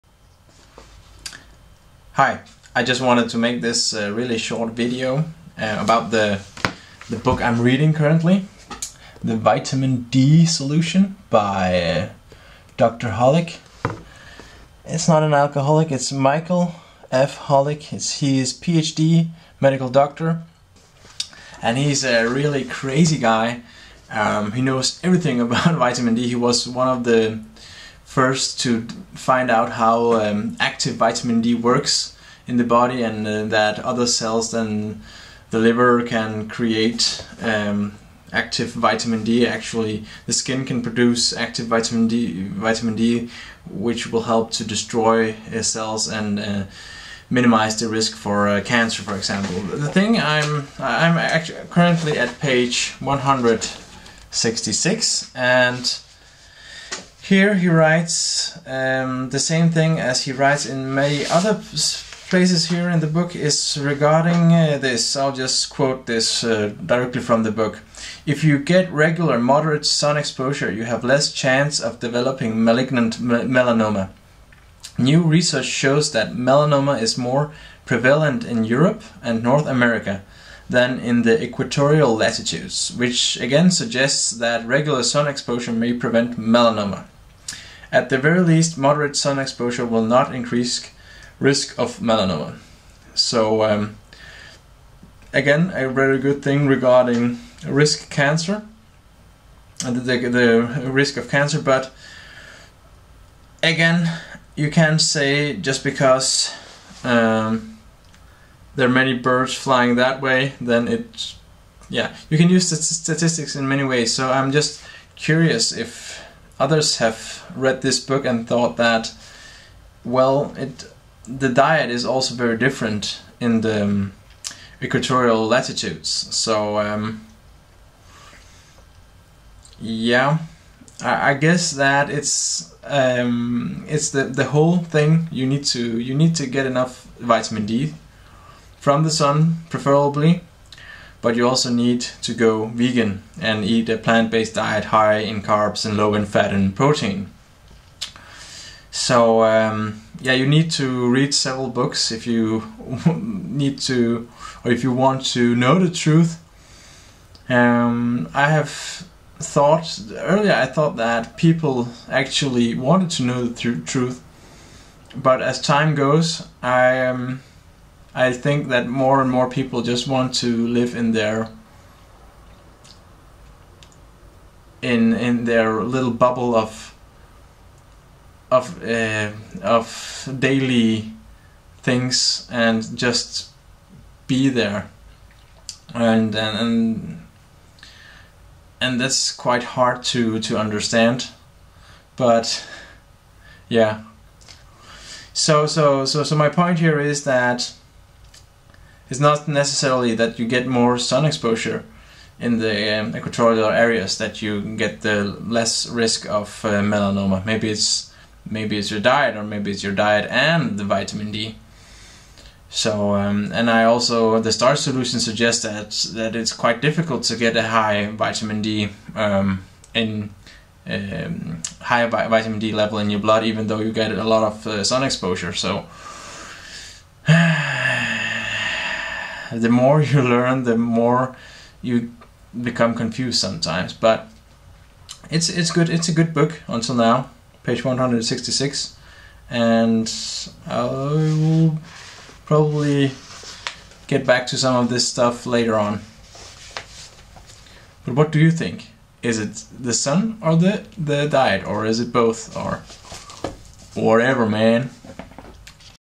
The Vitamin D Solution
The reductionist science prevails  in all paradigms. It is scary and we need to go towards the holistic aka wholelistic way of thinking. Will we ever understand, do we need to understand, and do we need to be able to explain all the mechanics, small parts and bits?

Do we really believe that Vitamin D alone will cure all our problems?